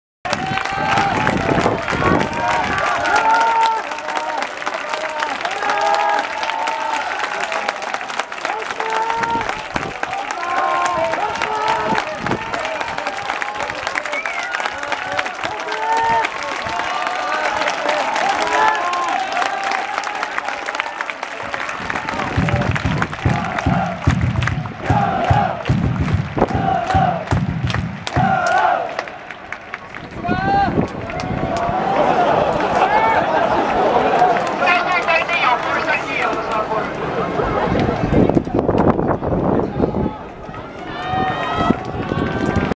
160512試合後、戸島章とキャラがゼルサポへ挨拶！
2019年5月12日〔日〕
2019年J2リーグ 第13節 ニッパツ三ツ沢球技場
横浜 FCvsFC町田ゼルビア

試合後、戸島章とキャラ（カルフィン・ヨン・ア・ピン）がFC町田ゼルビアサポーターへ挨拶！